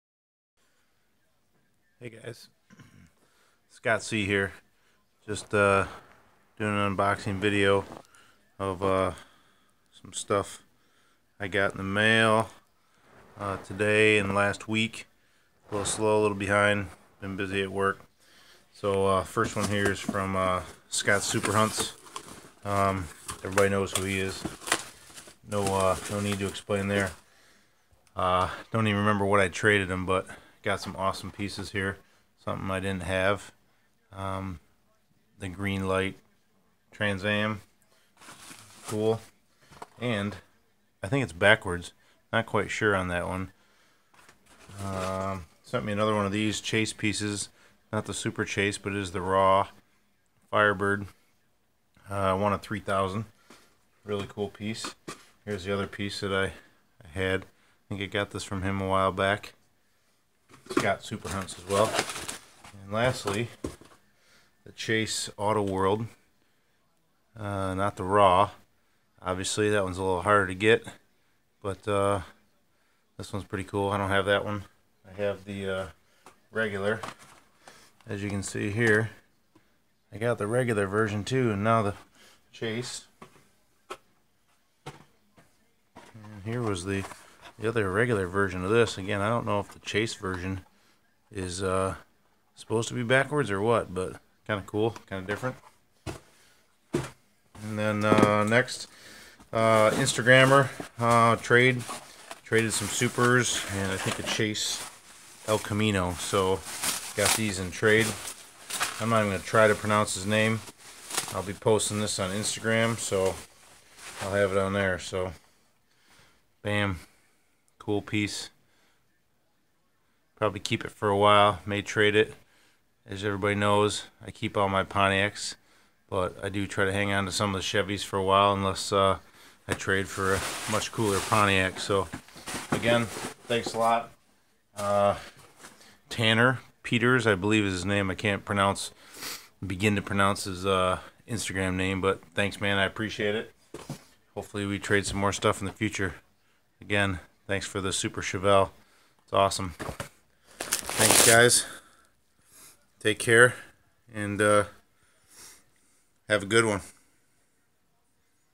Hot wheels super, chase, green machine, m2, thanks
Thanks to Scotts super hunts and to Tanner Peters for these cool trades. I'm happy to add these to my collection of Pontiacs that never get traded away. thanks again guys.